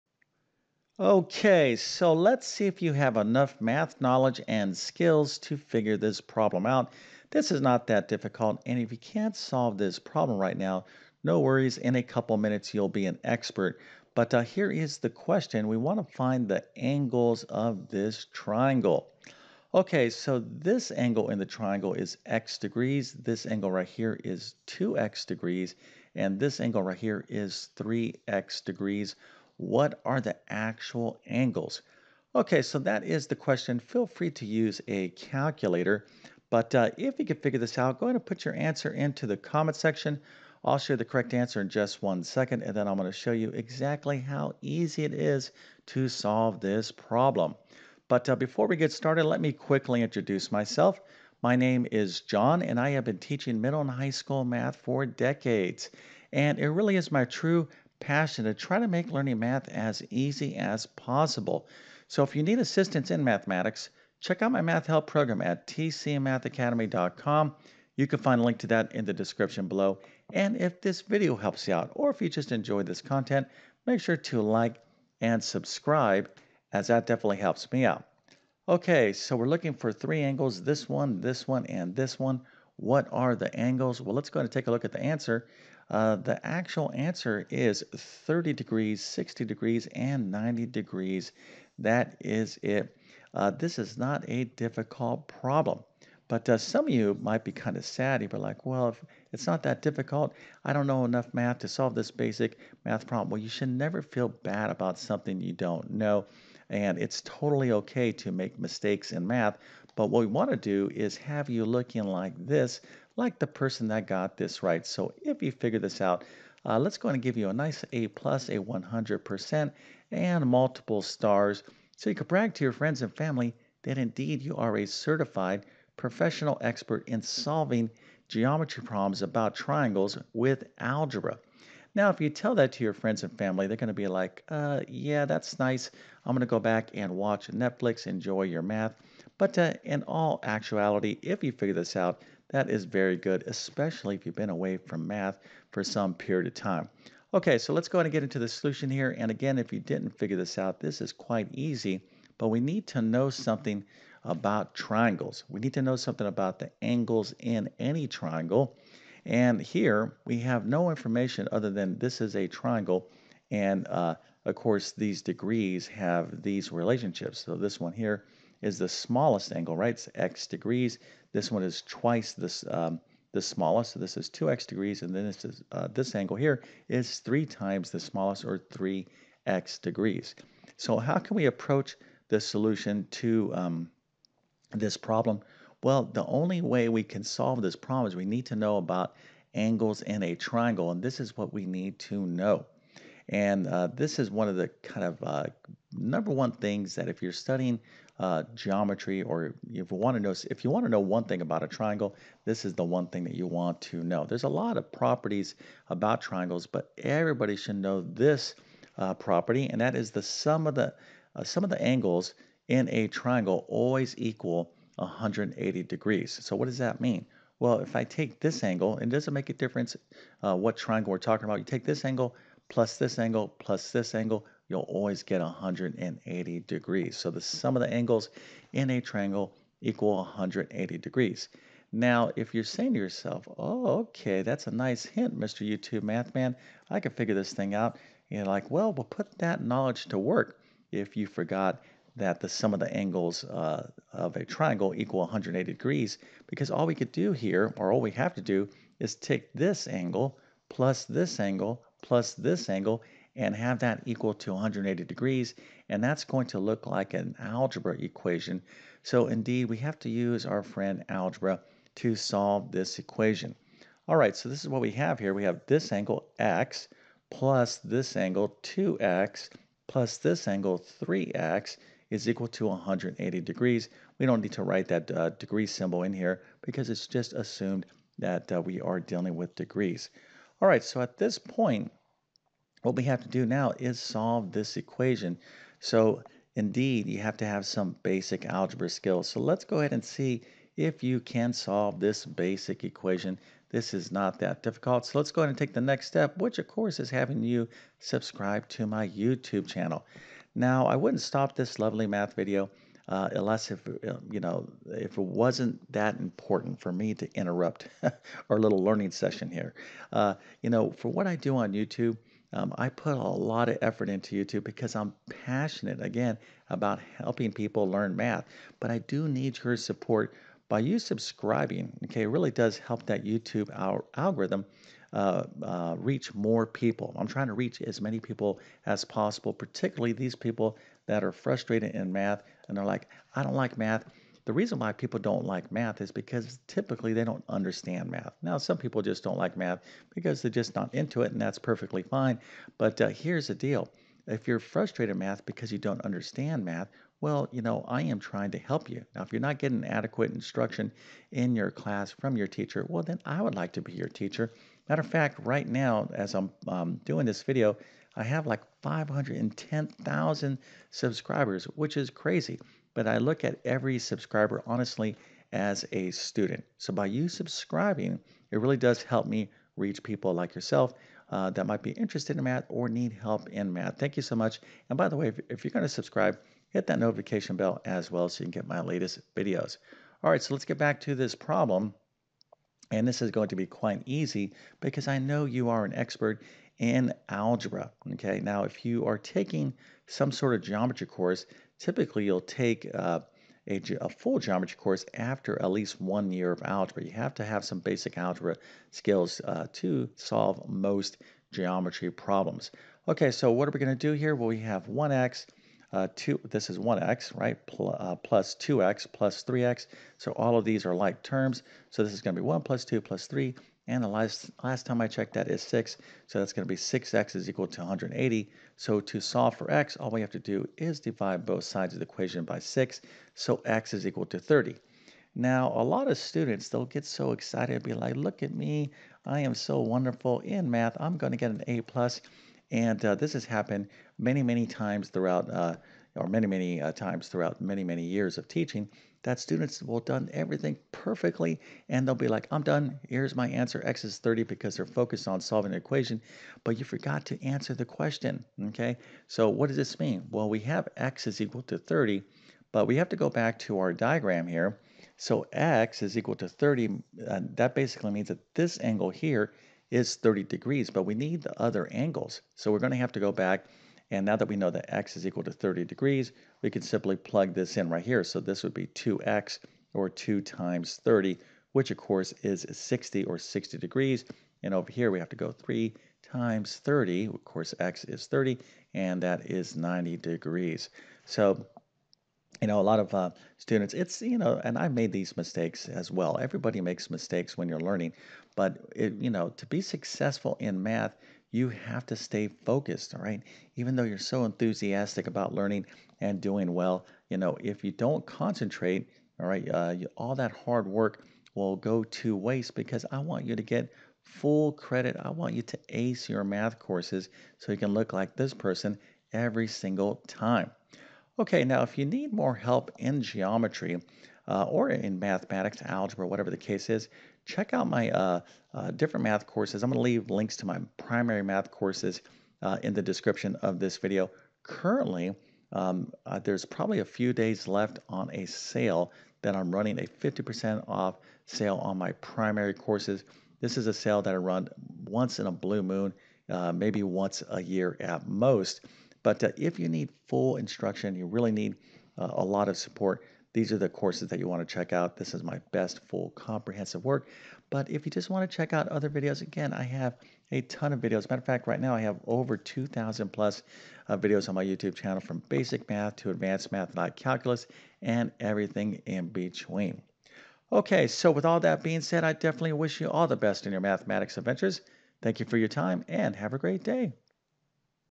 The angles of a triangle are x, 2x and 3x degrees, what are the angles?
How to find the missing angles in a triangle.